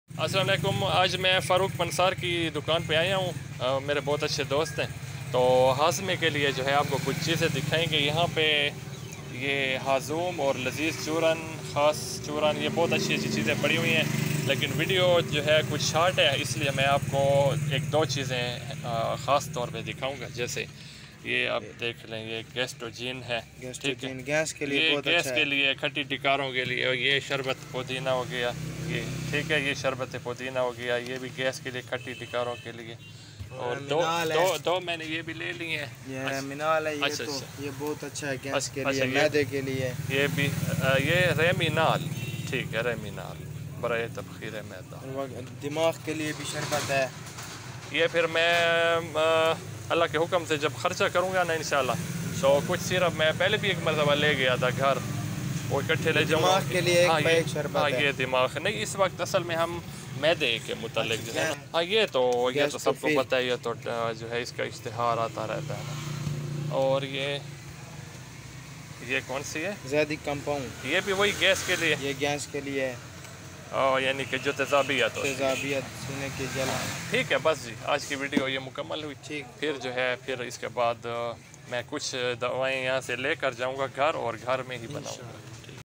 Maida ki gas ka ilaj Maida ki jalan our sozish ka ilaj badhazmi/ khatti dakaren take for stomic
Badhazmi Gase our maiday ki jalan#Acidity#Tezabiat#Acidity and Gase problems#Acid reflux treatment#Maida ki gas ka ilaj#vlog